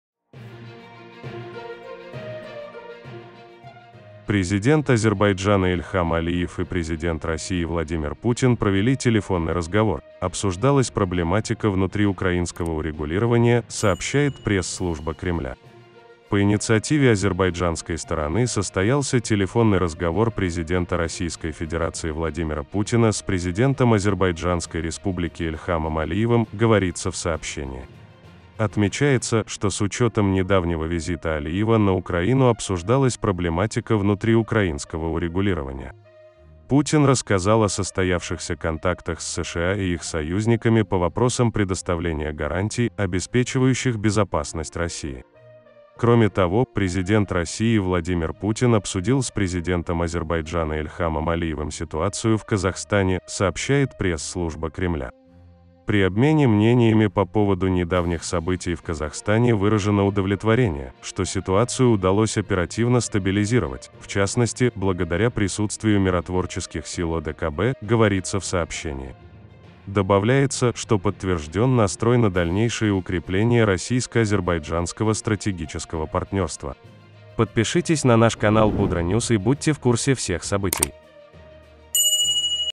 Путин и Алиев обсудили Украину и Казахстан
📰Президент Азербайджана Ильхам Алиев и президент России Владимир Путин провели телефонный разговор, обсуждалась проблематика внутриукраинского урегулирования, сообщает пресс-служба Кремля.

"По инициативе азербайджанской стороны состоялся телефонный разговор президента Российской Федерации Владимира Путина с президентом Азербайджанской Республики Ильхамом Алиевым", - говорится в сообщении.

Отмечается, что с учетом недавнего визита Алиева на Украину обсуждалась проблематика внутриукраинского урегулирования, . Путин рассказал о состоявшихся контактах с США и их союзниками по вопросам предоставления гарантий, обеспечивающих безопасность России.

Кроме того, президент России Владимир Путин обсудил с президентом Азербайджана Ильхамом Алиевым ситуацию в Казахстане, сообщает пресс-служба Кремля.

"При обмене мнениями по поводу недавних событий в Казахстане выражено удовлетворение, что ситуацию удалось оперативно стабилизировать, в частности, благодаря присутствию миротворческих сил ОДКБ", - говорится в сообщении.

Добавляется, что подтвержден настрой на дальнейшее укрепление российско-азербайджанского стратегического партнерства.